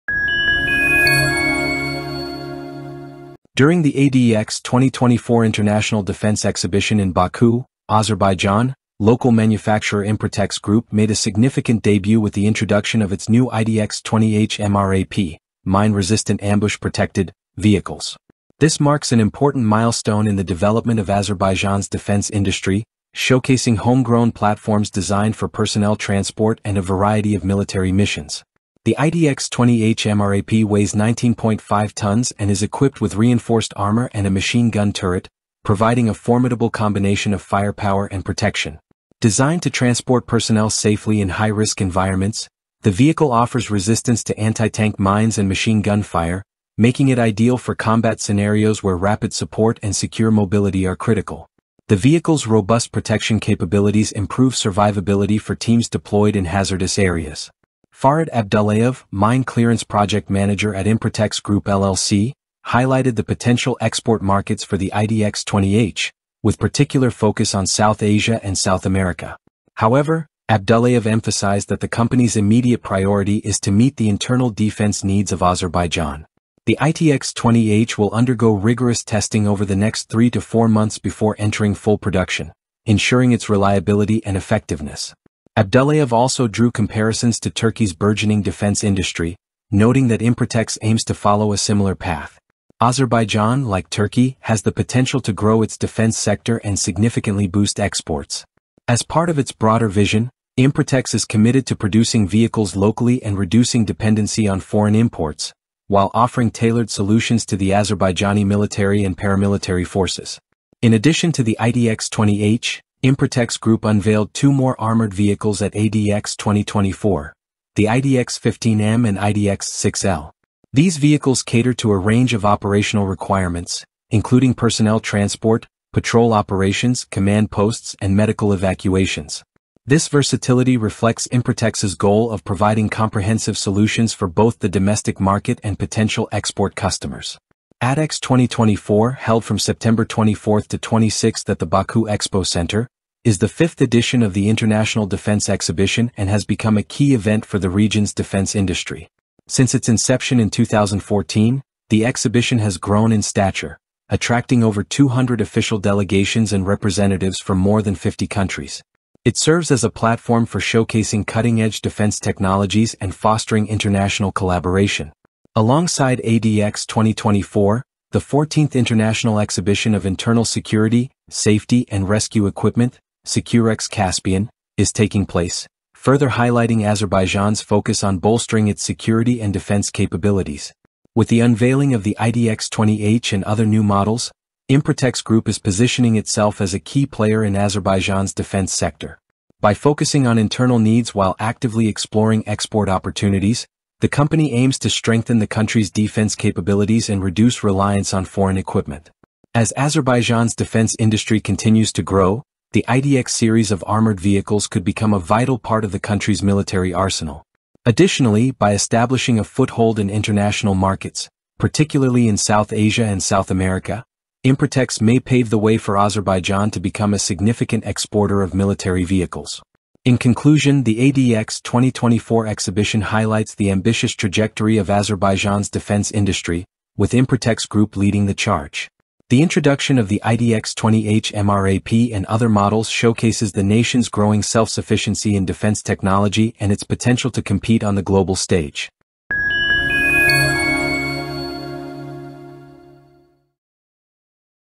First Presentation of ITX 20 H MRAP Armored Vehicle by Improtex Group at ADEX 2024
During the ADEX 2024 International Defense Exhibition in Baku, Azerbaijan, local manufacturer Improtex Group made a significant debut with the introduction of its new ITX-20 H MRAP (Mine Resistant Ambush Protected) vehicles. This marks an important milestone in the development of Azerbaijan’s defense industry, showcasing homegrown platforms designed for personnel transport and a variety of military missions.